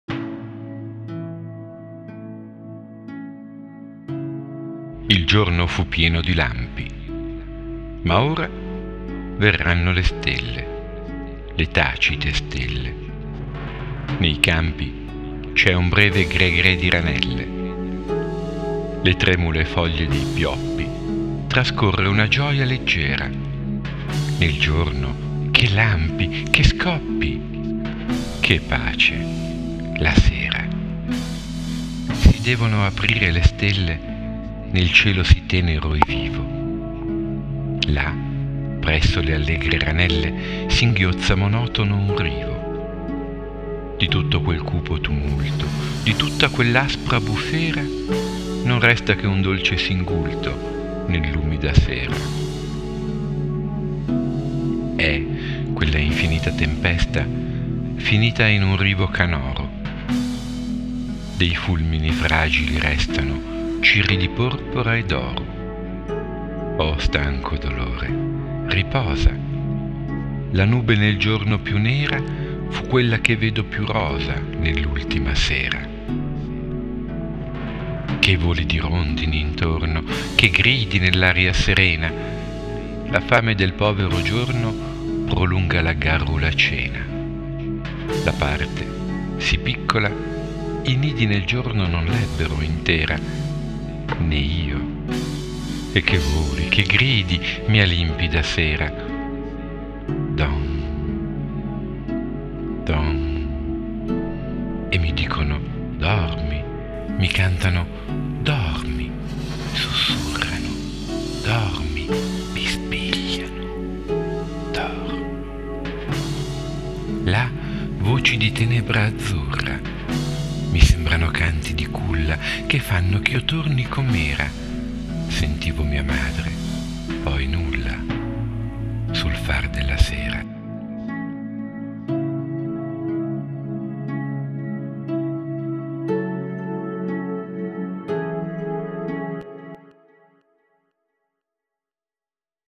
La mia sera, Giovanni Pascoli. Lettura e musica di Alberto Pian
Lettura con base musicale leggi il blog, scarica il Magazine, ottieni altri contenuti.